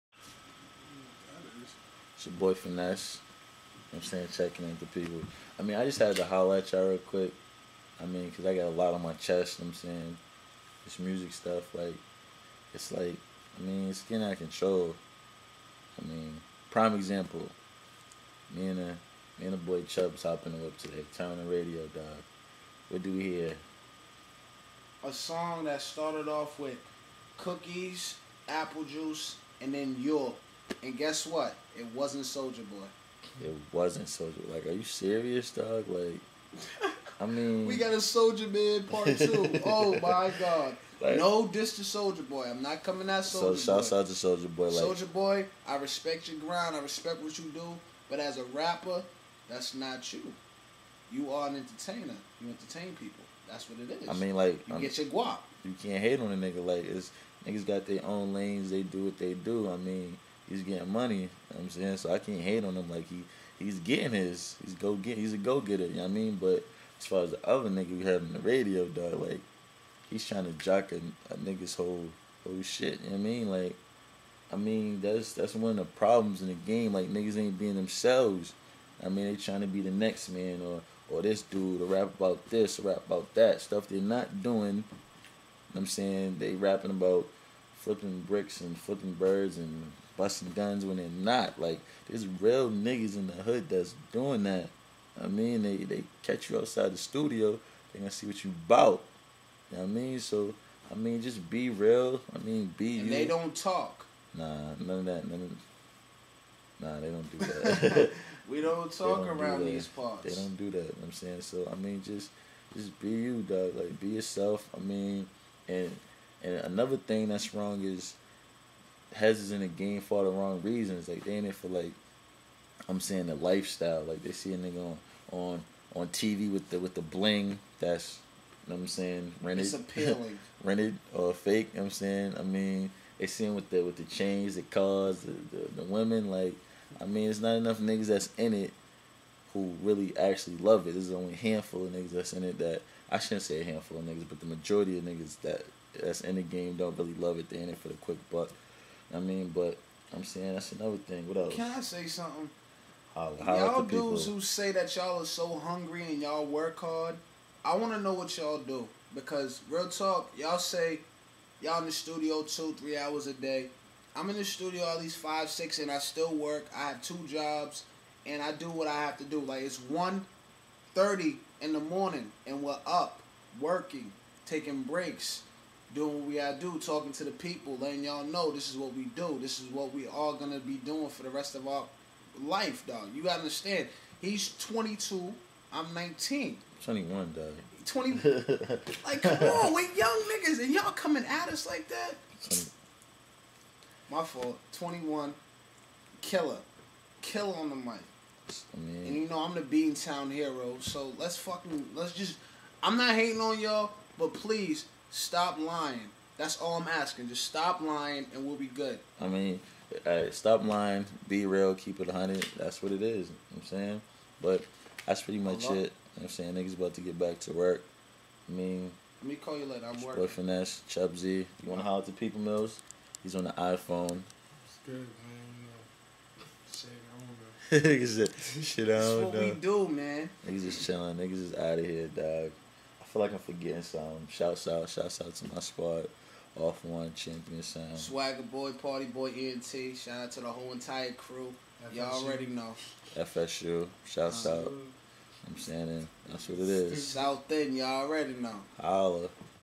Phaness speaks about the rap game
Phaness and Chubb-Z speaking about the rap game